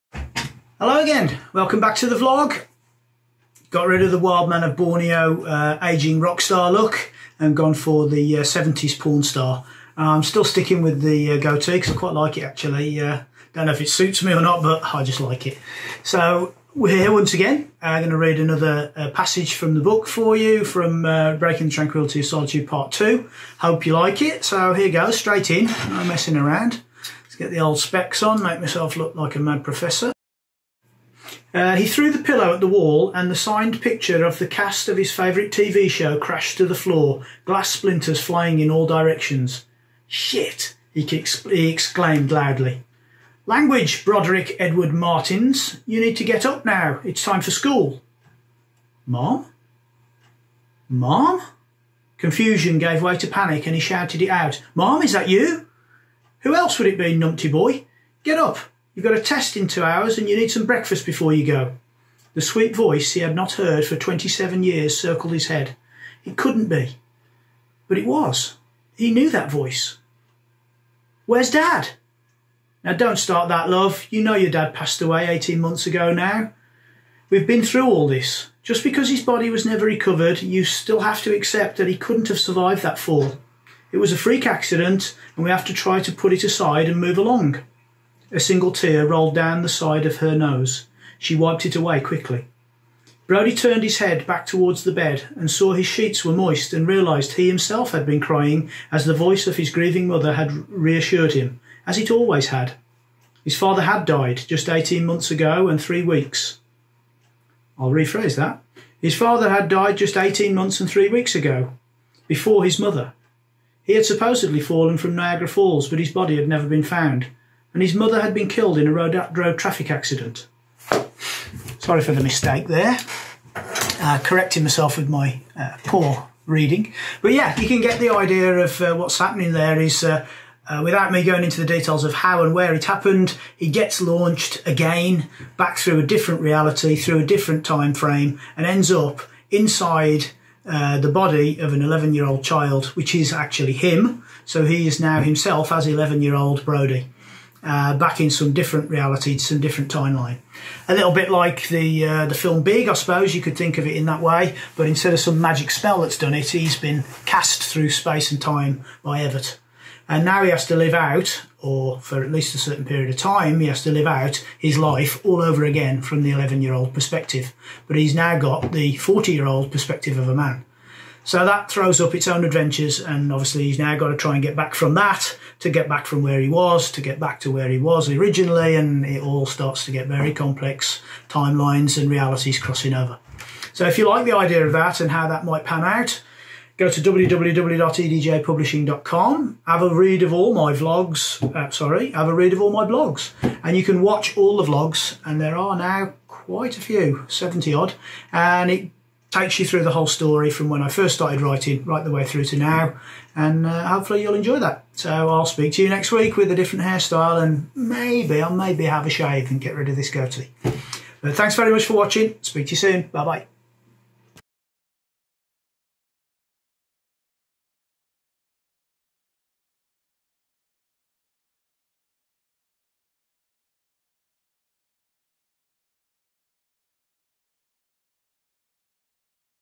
Breaking The Tranquillity of Solitude - Vlog 71 - Mom, Is That You?
Part Two of Breaking The Tranquillity of Solitude is out now and here's Author Eddie Johnson with another excerpt from book 2 and the moment Brody finds himself back in time with someone familiar.

Vlog from author Eddie Johnson. Get his book Breaking The Tranquillity of Solitude Part Two from AMAZON now.